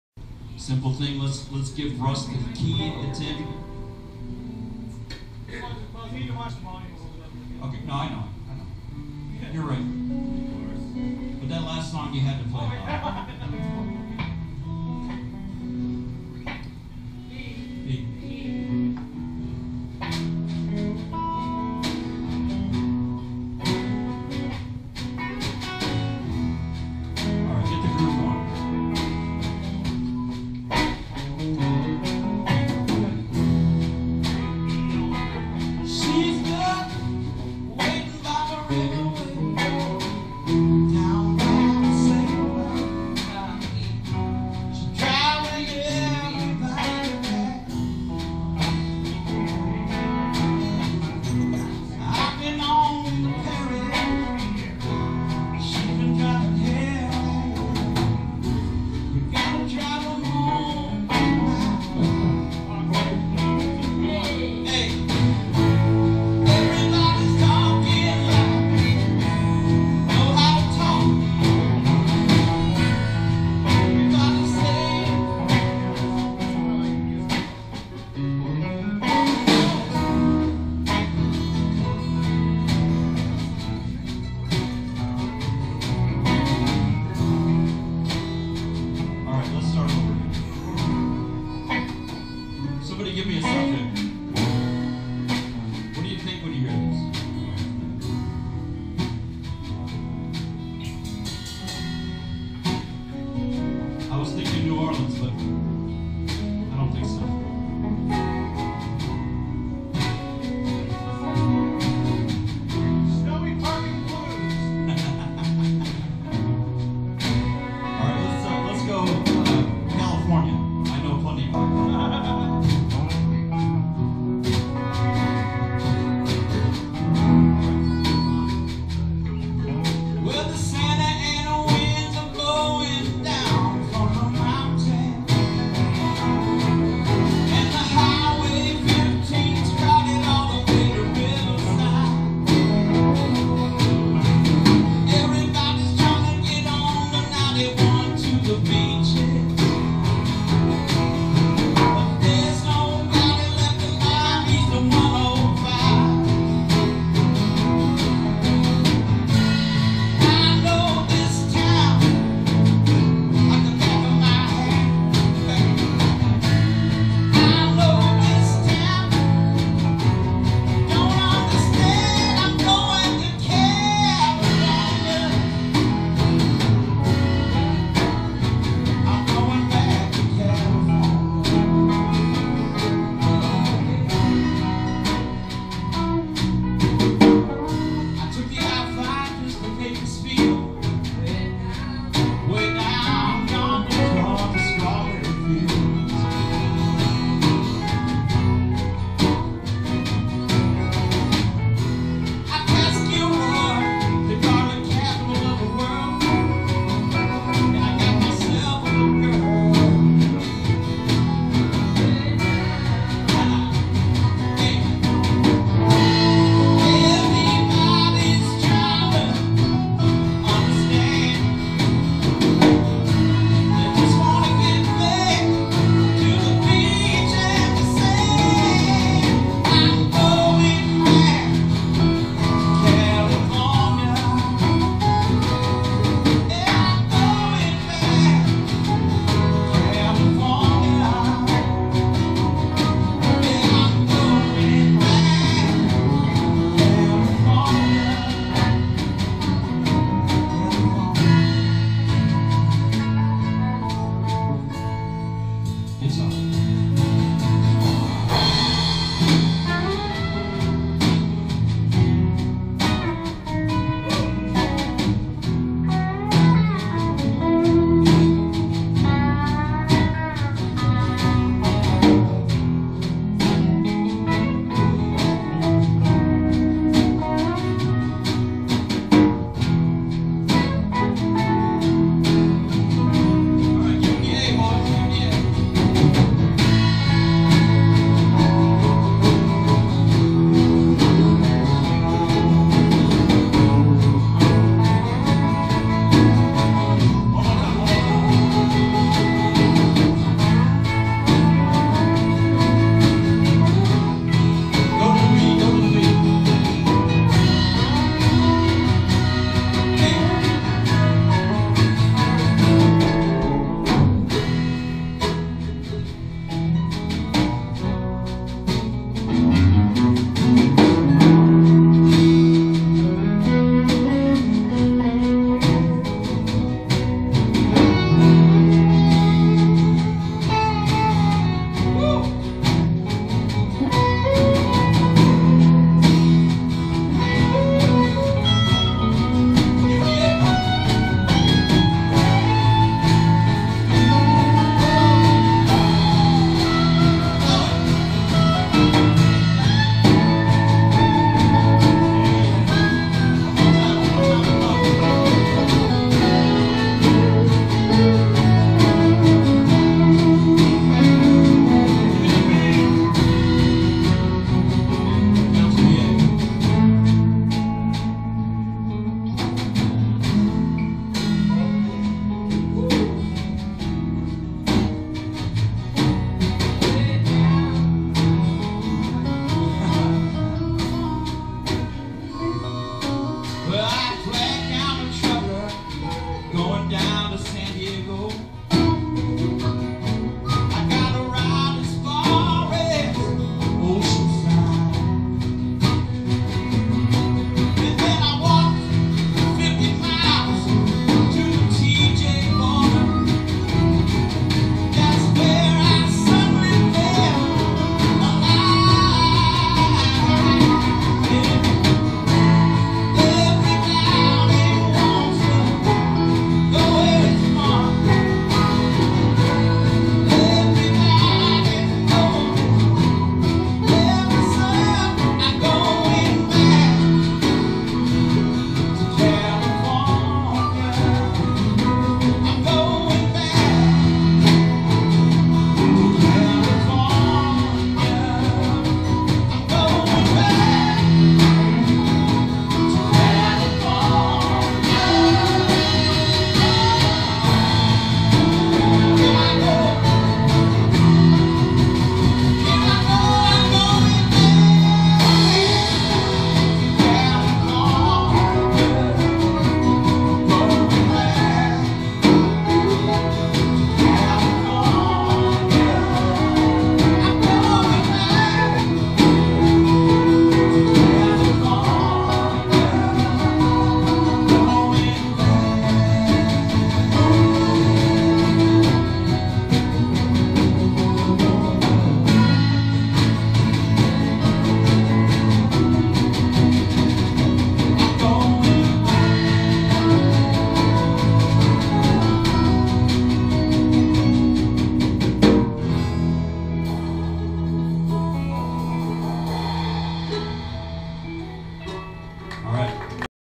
Blues Music & Jams
Corner Coffee Sat. Morning Jam on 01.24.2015
Videos made using GoPro + Yoby tripod